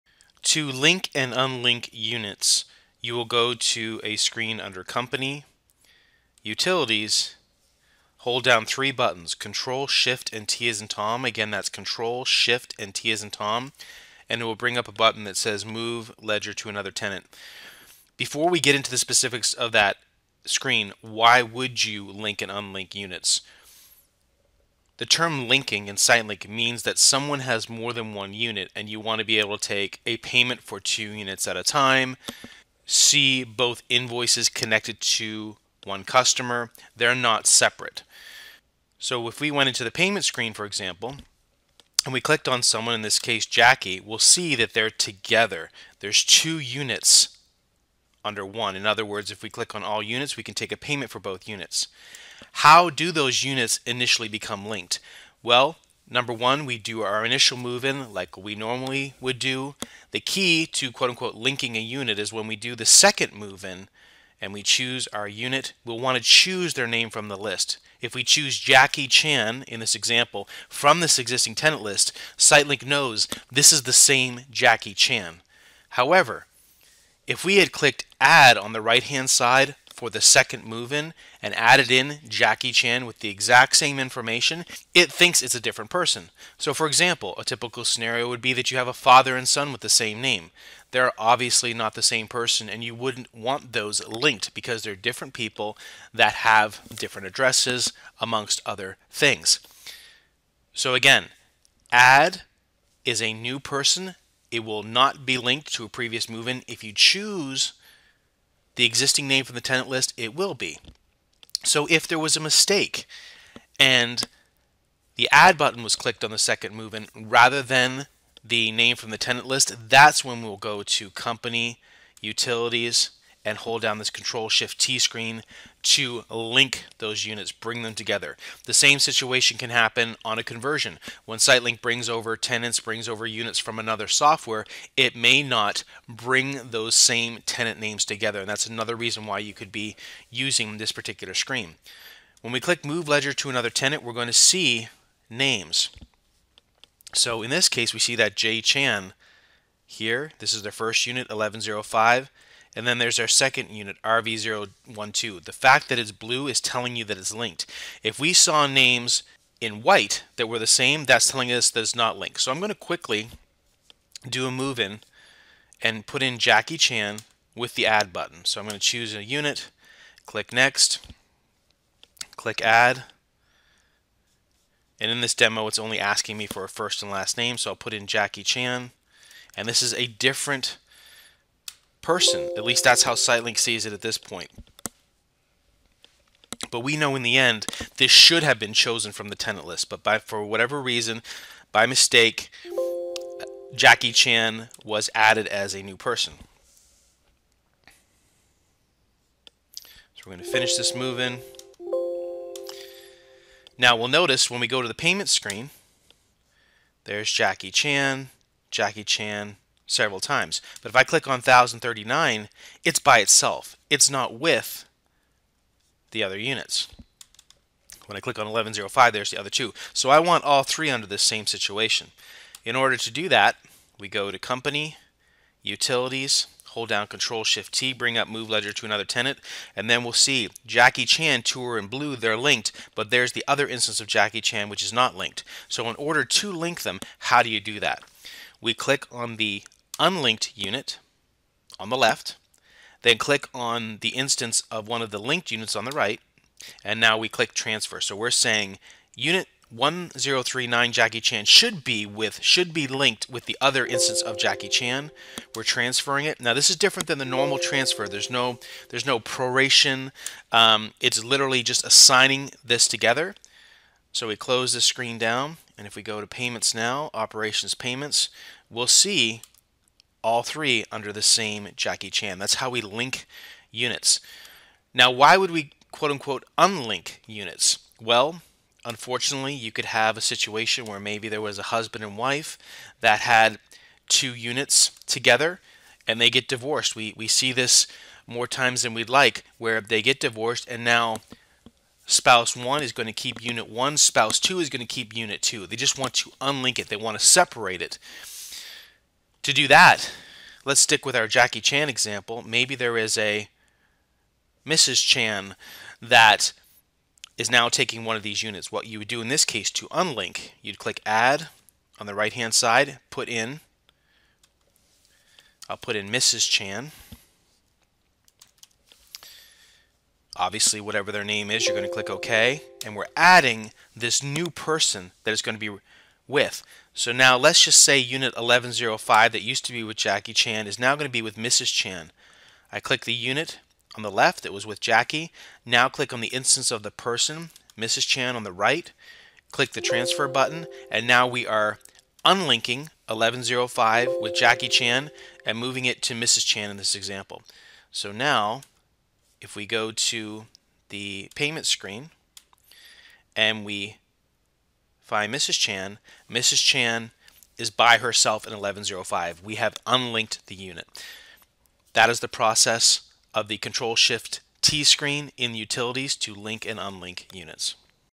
Linking and Unlinking - SiteLink Training Video
Learn how to link and unlink units using your SiteLink Smart Management Software.